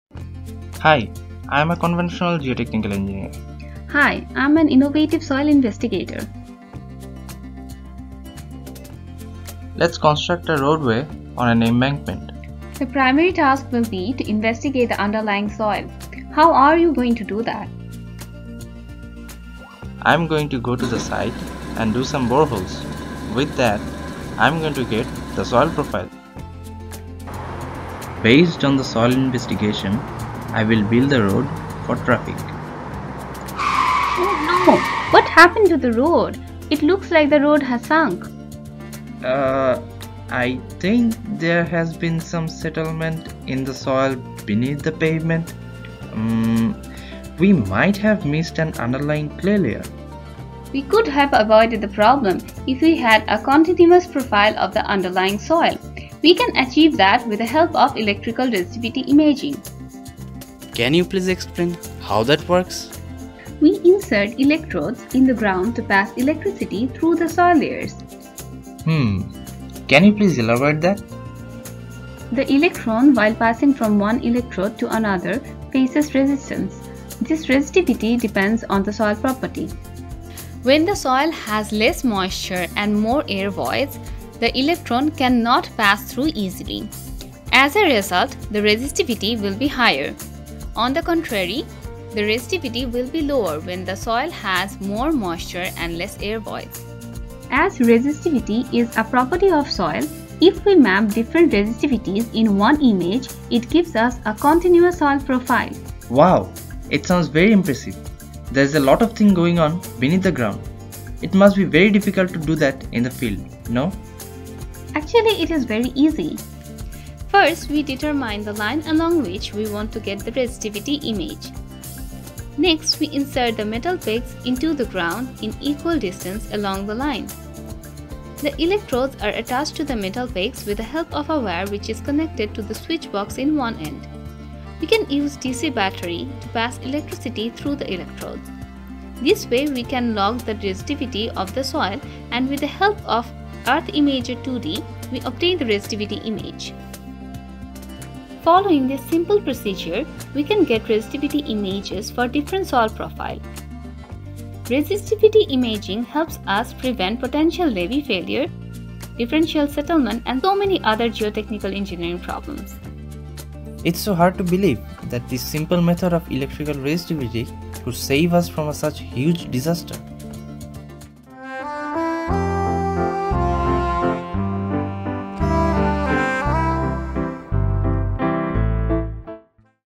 Electrical Resistivity Imaging | The University of Texas at Arlington
The objective of the Geo-Video competition is to develop short videos explaining various geotechnical concepts that could be used in classrooms at various levels (elementary school through college) and by the lay person to understand geotechnical principles.

"Electrical Resistivity Imaging" is the first place video in the 2023 competition. It was created by a team from University of Texas at Arlington.

Video created by Md Lutfor Rahman and Niloy Gupta
Advisor: Dr. Md Sahadat Hossain

The Geo-Institute is a technical society with about 12,000 geotechnical engineers and geologists as members.